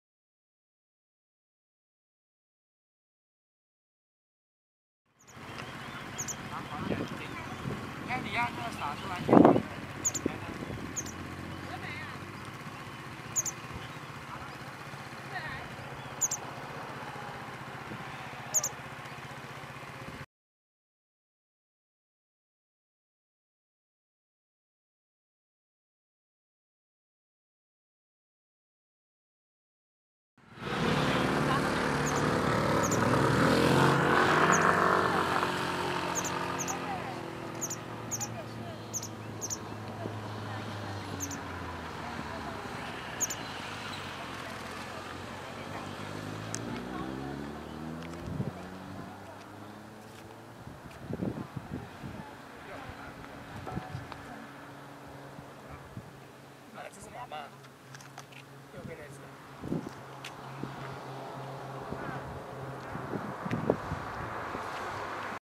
20150914金翼白眉@小風口 White-whiskered Laughingthrush
台灣噪眉,台灣特有種
兩隻金翼白眉在合歡山昆陽停車場,一隻可能是幼鳥,不斷振翅鳴叫,跟著親鳥走動,等到親鳥靠近時,立刻轉身背對,翹起屁屁,是想要親鳥來處理poopoo嗎?